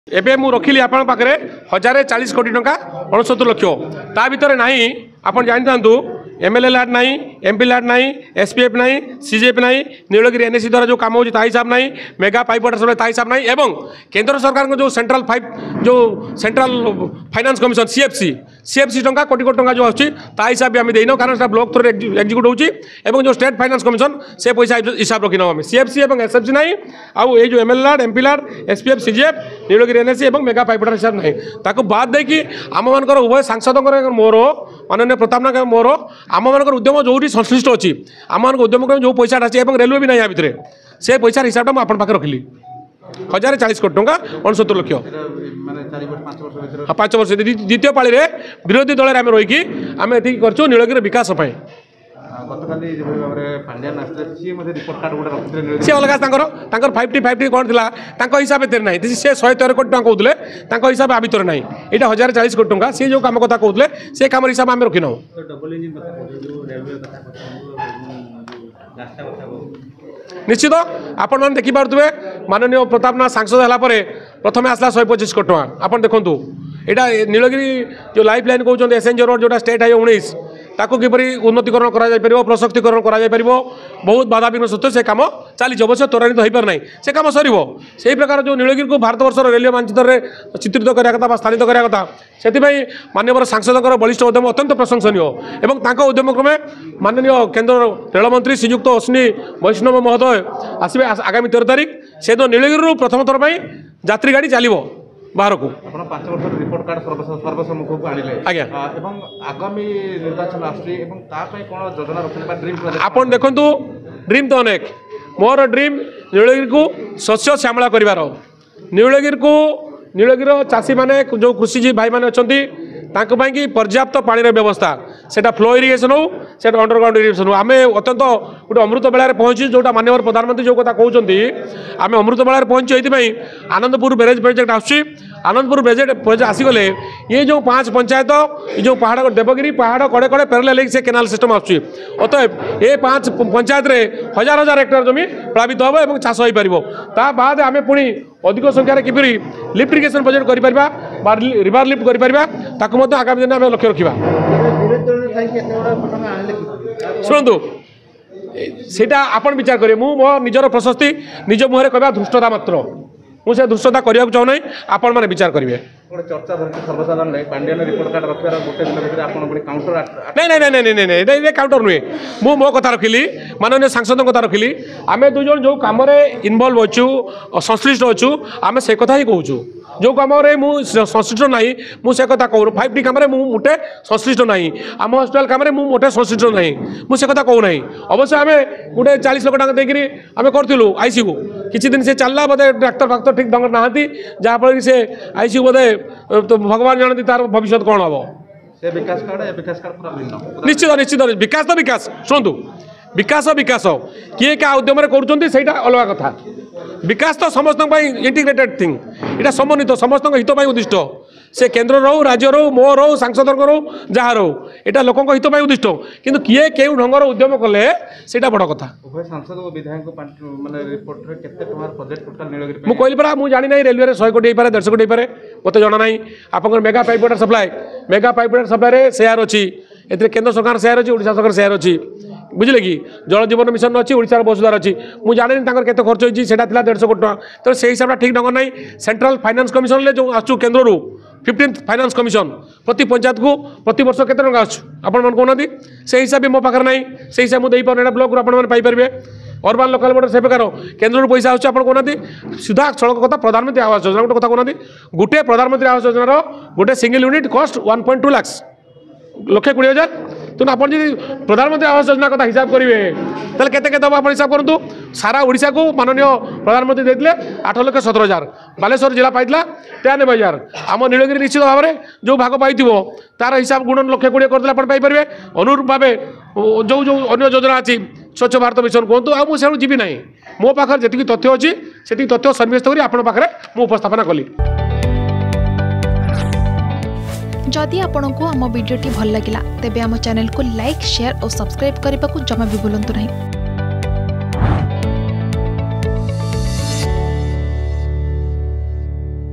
BJP's Press Conference On The Development Of Nilagiri || Balasore || Argus News
BJP's Press Conference On The Development Of Nilagiri